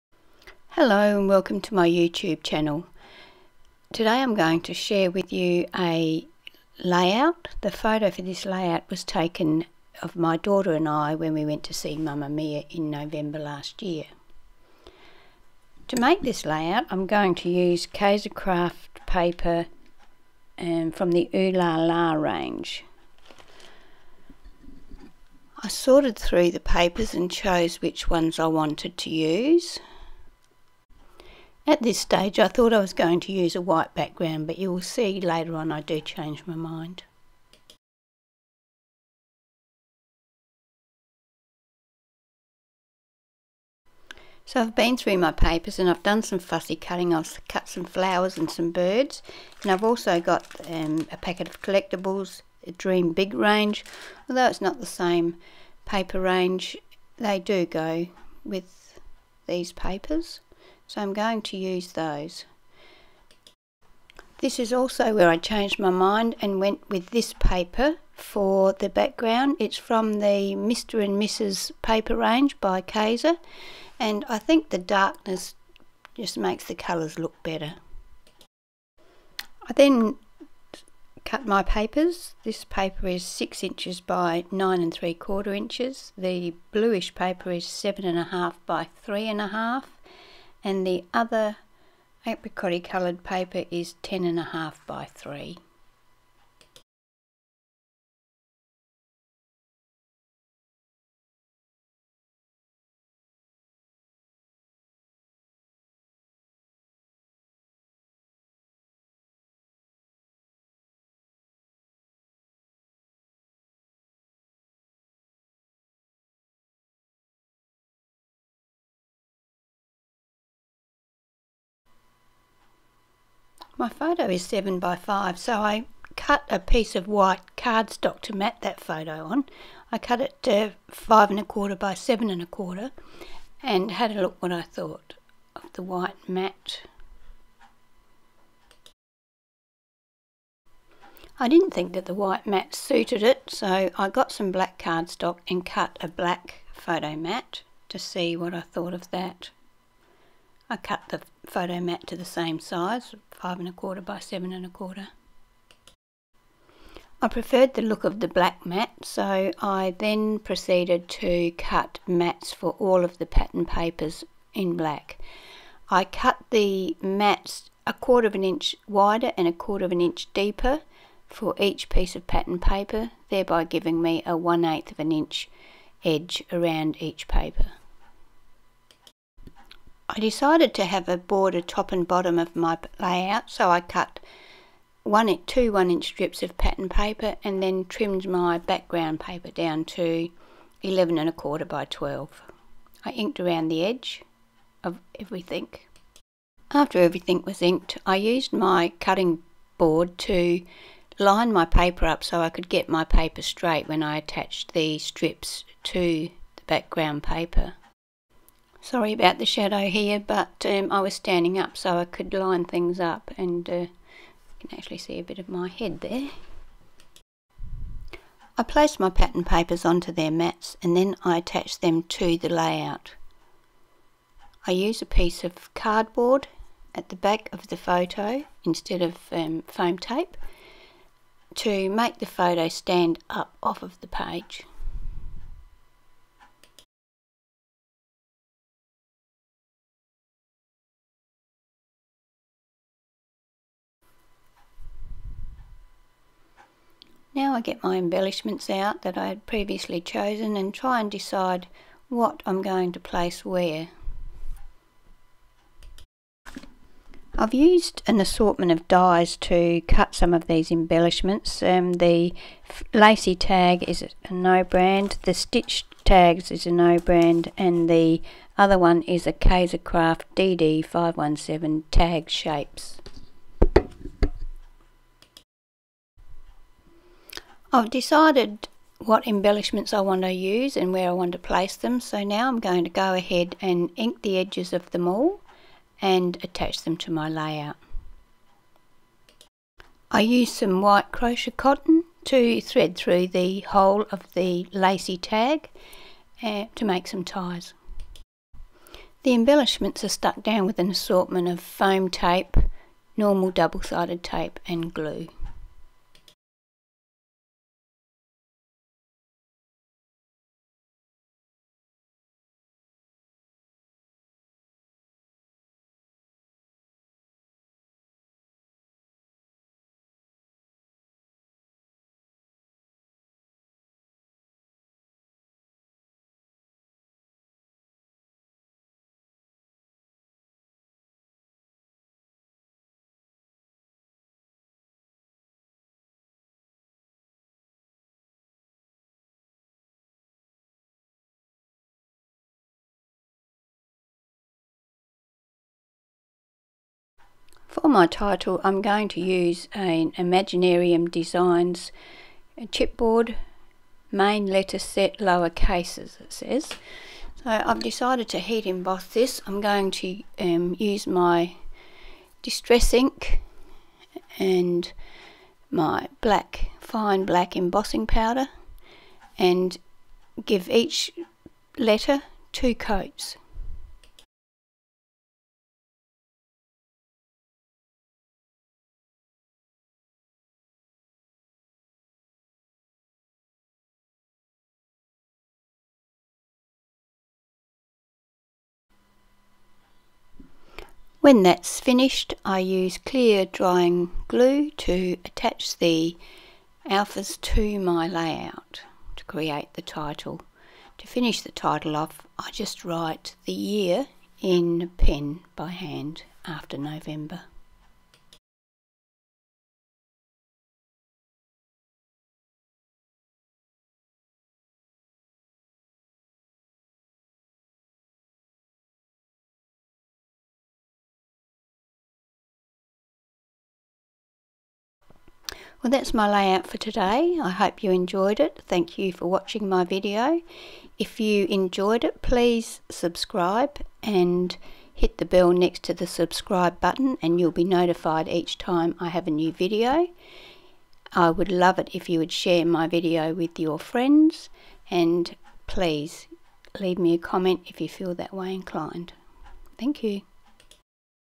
Mamma Mia Layout
A layout using Kaisercraft papers.. I have used die cuts, fussy cutting and heat embossed chipboard on this layout.

You can see more of my layouts here:-